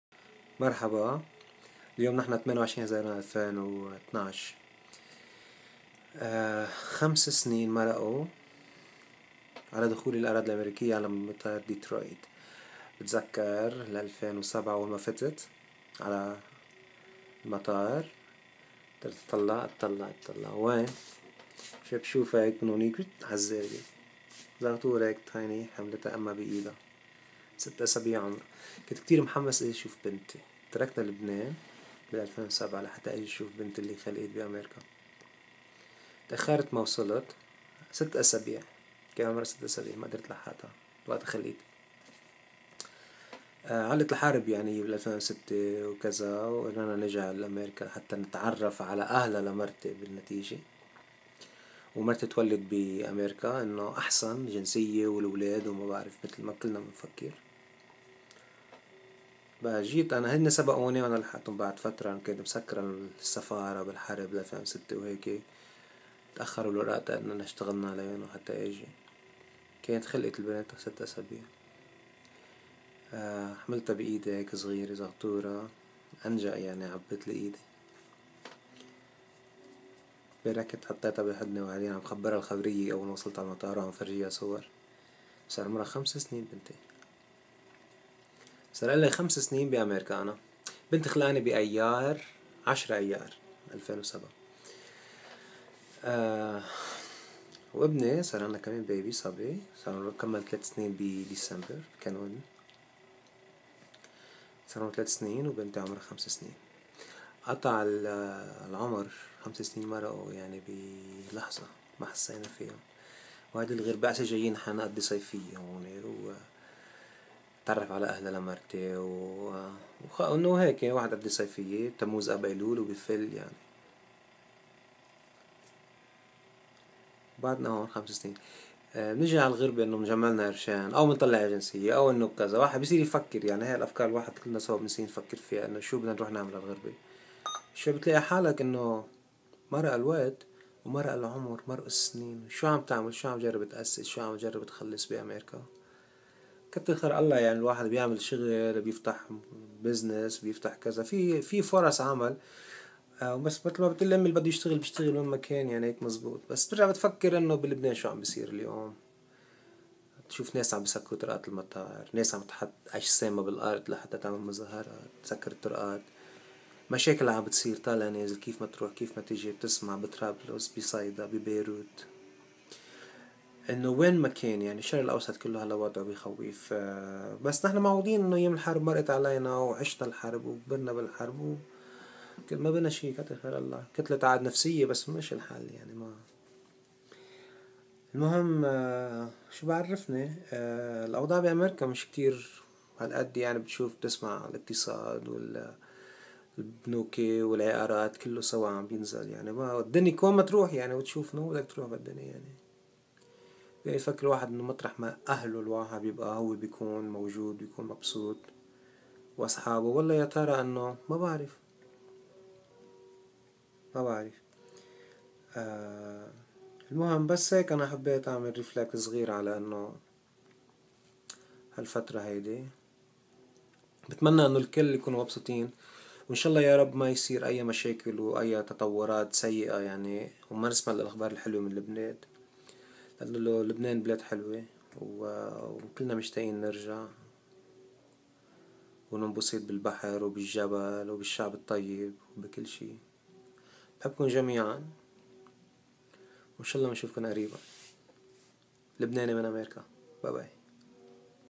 Lebanese in America - 5 yrs anniversary
5 years ago I landed on the American soil.
Thoughts and reflections